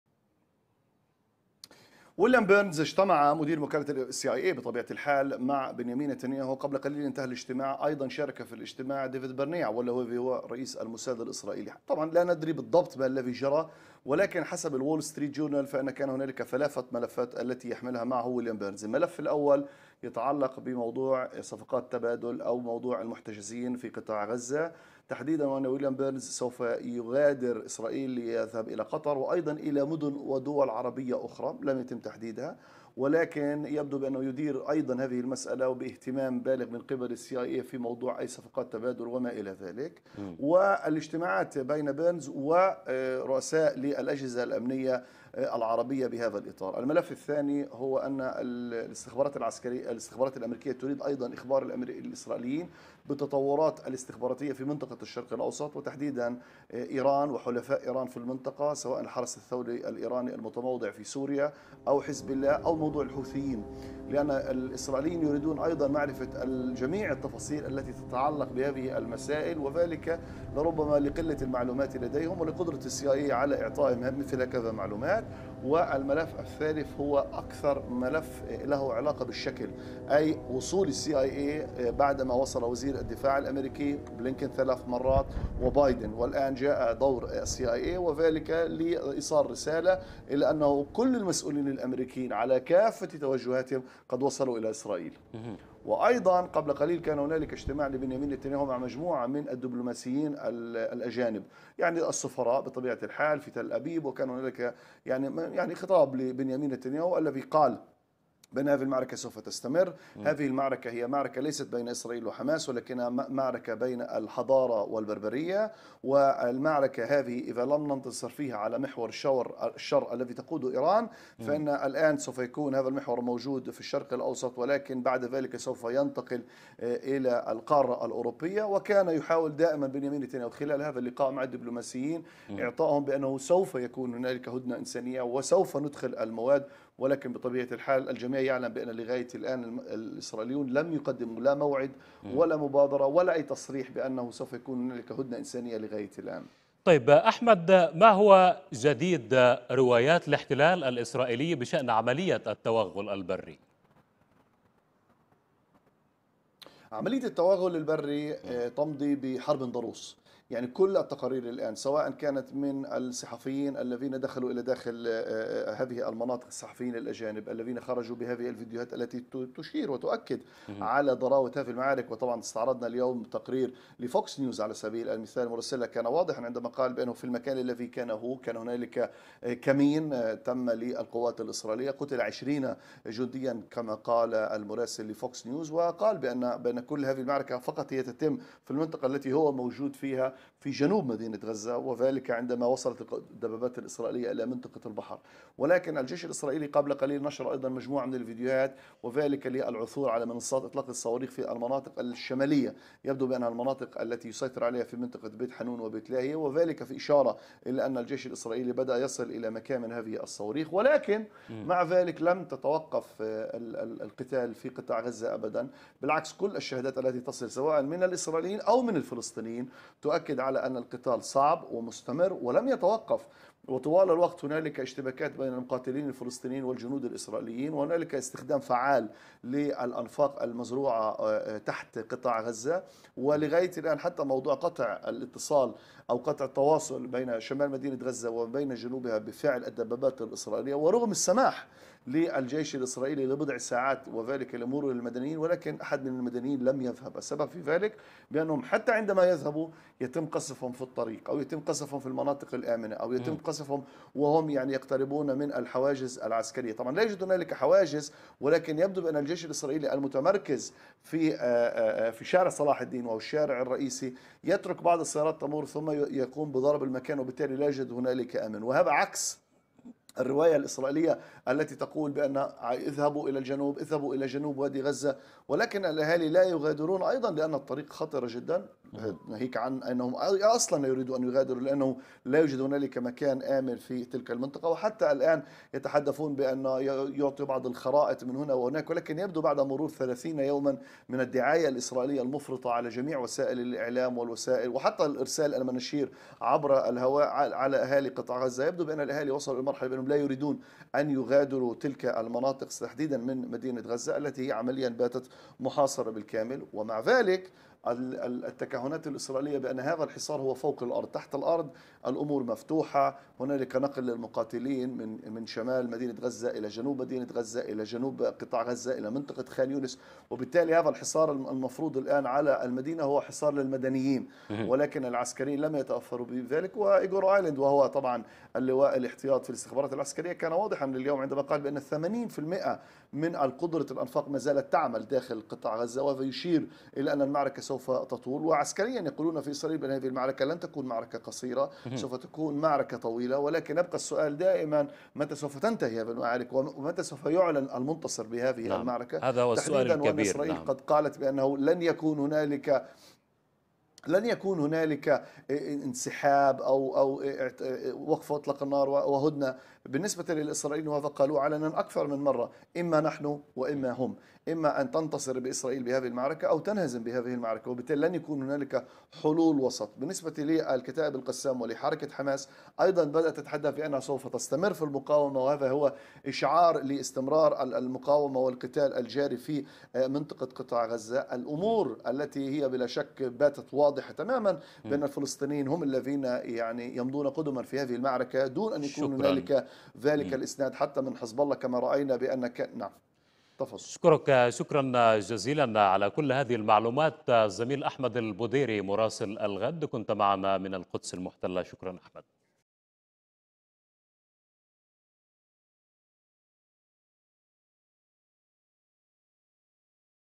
مدير CIA يبحث مع الإسرائيليين ملف تبادل الأسرى ليلتقي لاحقا مع نظرائه العرب
مستمرون في تغطيتنا من قناة الغد للعدوان الإسرائيلي على قطاع غزة، حيث شن جيش الاحتلال الإسرائيلي غارات على شمال قطاع غزة هي الأعنف منذ بدء العدوان.
ونفذ الاحتلال حزاما ناريا وقصف عدة مناطق بالفسفور الأبيض المحرم دوليا، بينما انقطعت شبكة الاتصالات والانترنت عن قطاع غزة .
وأفاد مراسلنا بأن جيش الاحتلال قصف منزلا في معسكر جباليا شمالي القطاع، كما استهدفت غارات الاحتلال محيط مبنى مستشفى القدس بحي تل الهوا جنوبي مدينة غزة... وللمزيد من التفاصيل ينضم إلينامراسل الغد من القدس أحمد البديري مع الإعلامي حسين حسني